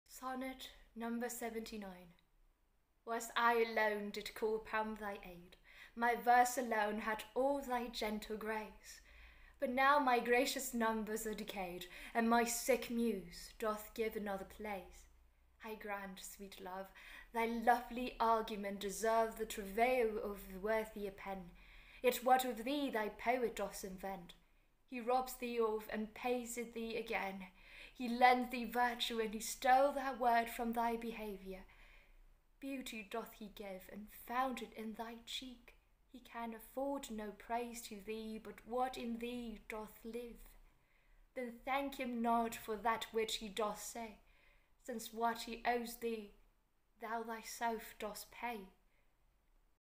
Shakespeare Sonnet Marathon #79
Whilst I alone did call upon thy aid
My verse alone had all thy gentle grace;
But now my gracious numbers are decayed,
And my sick Muse doth give another place.
I grant, sweet love, thy lovely argument
Deserves the travail of a worthier pen;
Yet what of thee thy poet doth invent
He robs thee of, and pays it thee again;
He lends thee virtue, and he stole that word
From thy behavior; beauty doth he give,
And found it in thy cheek; he can afford
No praise to thee, but what in thee doth live.
Then thank him not for that which he doth say,
Since what he owes thee, thou thyself dost pay.

Quoted from Internet Shakespeare Edition